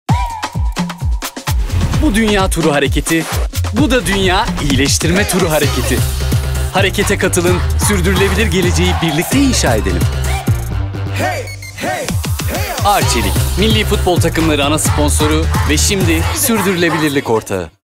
DünyaİyileştirmeTuru X Emin Bayram
Emin Bayram DünyaİyileştirmeTuru hareketini yaptı, sürdürülebilir bir gelecek için adım attı.

Sen de katıl, DünyaİyileştirmeTuru hareketini evinde başlat.